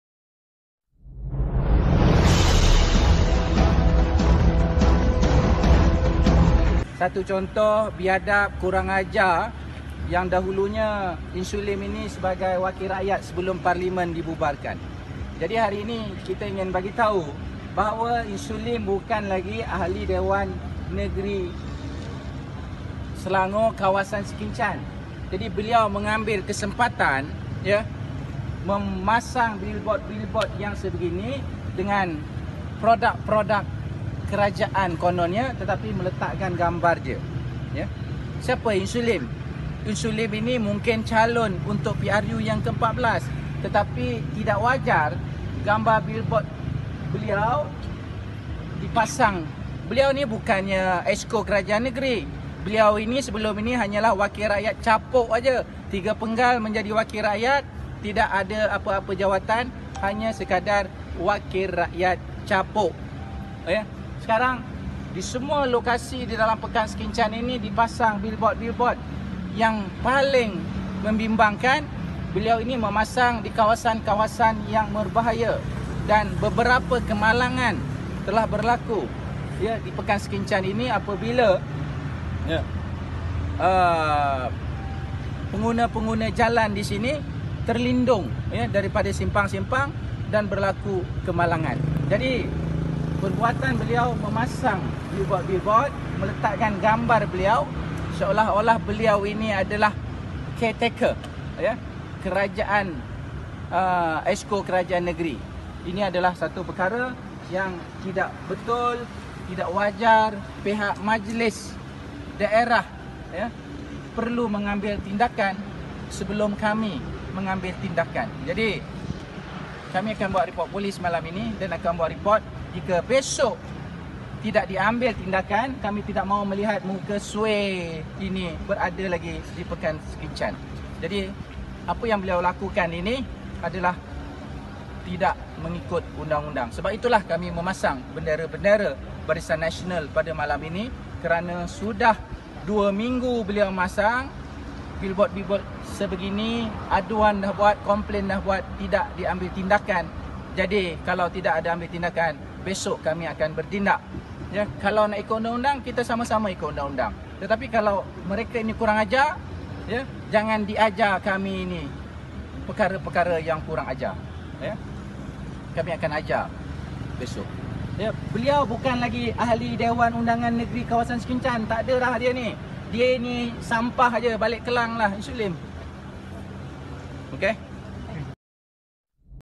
GEMPAR AMARAN BUAT SUEE LIM : "SAYA AKAN ROBOHKAN SENDIRI" - JAMAL
Satu sidang media gempar telah diadakan oleh Ketua Bahagian UMNO Sungai Besar, Datuk Seri Jamal Md Yunos berhubung tindakan bekas Ahli Dewan Undangan Negeri (ADUN) Sekinchan, Ng Suee Lim yang memasang papan tanda kempen produk kerajaan Selangor yang dilihat bertujuan untuk mempopularkan dirinya.

Jamal turut mempersoalkan tindakan pihak Majlis Daerah Sabak Bernam (MDSB) yang tidak mengambil perhatian berkenaan perkara itu memandangkan Suee Lim bukan lagi ADUN Sekinchan.

Ekoran dari tindakan itu, beberapa laporan polis akan dibuat serta merta pada malam ini.

Suee Lim dilihat cuba bermain politik 'kotor' bagi meraih undi menjelang Pilihan Raya Umum ke-14 (PRU14) yang telah ditetapkan pada 9 Mei 2018.

 Oleh Hazierra Lukman